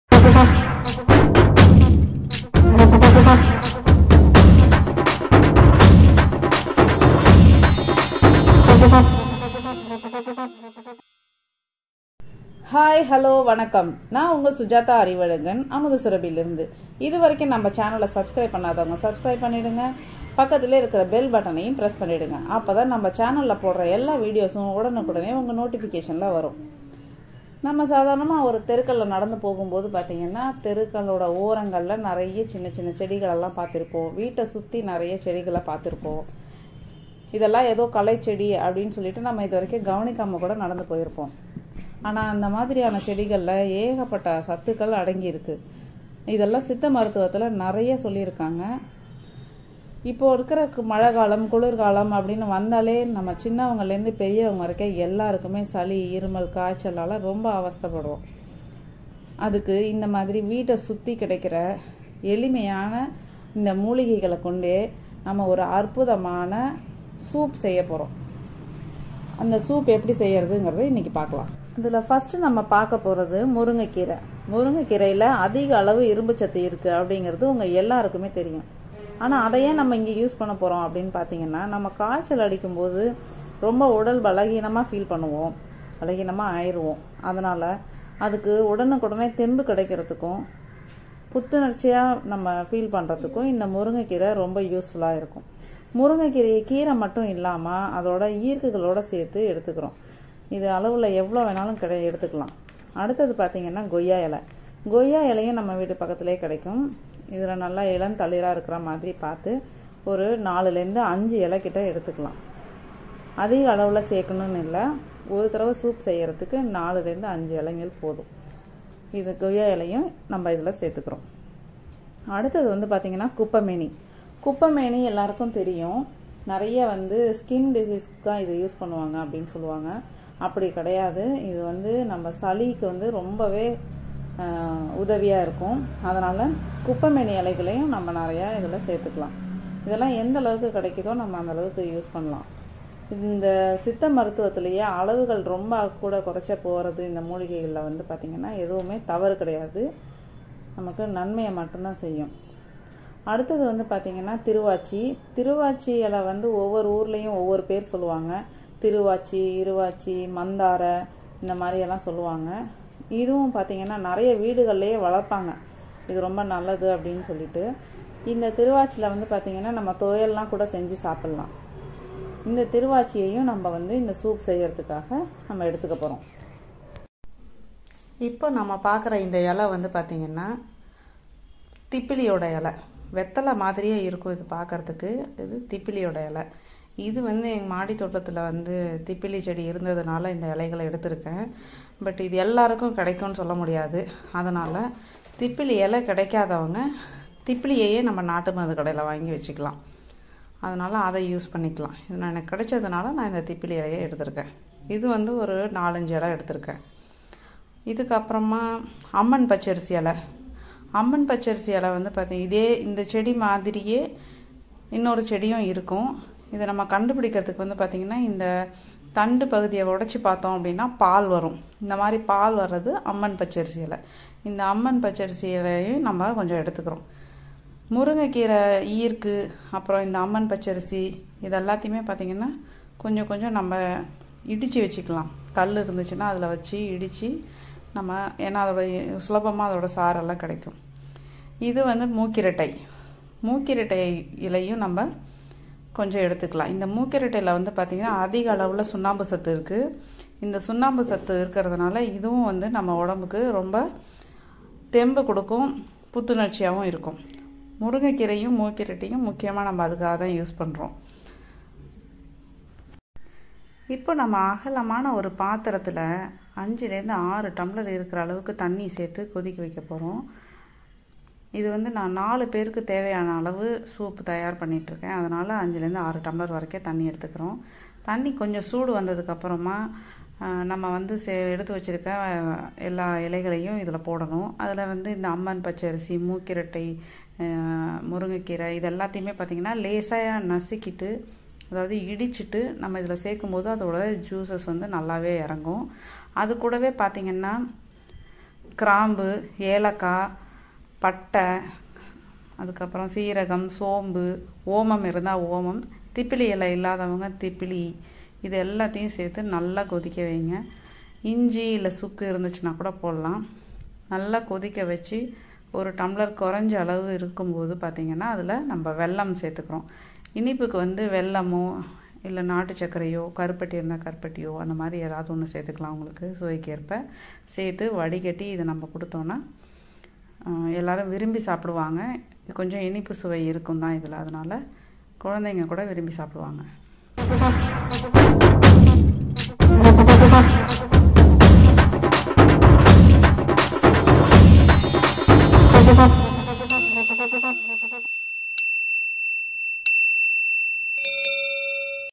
சளி,இருமல் மற்றும் காய்ச்சல் போக்கும் மூலிகை சூப் | சுஜாதா அறிவழகன் | Mooligai | Soup | Kashayam
மூலிகைகள் - முருங்கை கீரை,கொய்யா இலை, குப்பைமேனி,திப்பிலி,இருவாச்சி இலை,அம்மன் பச்சரிசி,மூக்கிரட்டை

இவற்றுடன் பட்டை,லவங்கம்,சோம்பு,சீரகம்,ஓமம்,மிளகு,இஞ்சி அல்லது சுக்கு சேர்த்து நன்கு கொதிக்க வைத்து, இறுதியில் தேவைக்கேற்ப வெல்லம் அல்லது நாட்டுச்சக்கரை அல்லது கருப்பட்டி அல்லது தேன் சேர்த்து வடிகட்டி பருகலாம்.